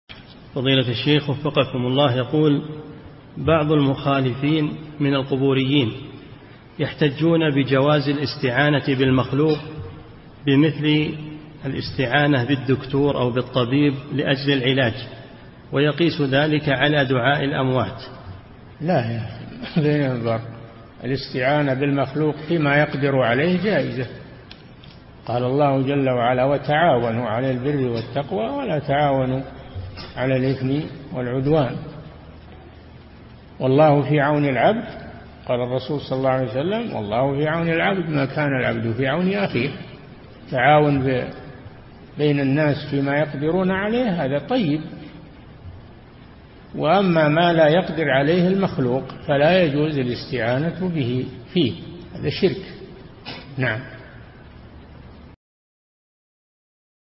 [732 -939] بعض المخالفين من القبوريين يحتجون بجواز الاستعانة بالمخلوق بمثل الاستعانة بالطبيب!
-[732 -939] السؤال: بعض المخالفين من القبوريين يحتجون بجواز الاستعانة بالمخلوق بمثل الاستعانة بالدكتور أو بالطبيب لأجل العلاج، ويقيس ذلك على دعاء الأموات؟
الجواب: لا يا أخي الاستعانة بالمخلوق فيما يقدر عليه جائزة قال الله تعالى {وتعاونوا على البر والتقوى ولا تعاونوا على الإثم والعدوان}  قال الرسول - صلى الله عليه وسلّم – «والله في عون العبد ما كان العبد في عون أخيه» التعاون بين الناس بما يقدرون عليه هذا طيب وأما ما لا يقدر عليه المخلوق فلا يجوز الاستعانة به فيه هذا شرك .
للاشتراك في قناة الشيخ صالح الفوزان

للاستفسار والإقتراحات والدعم
رئيس الفريق
لمشروع كبار العلماء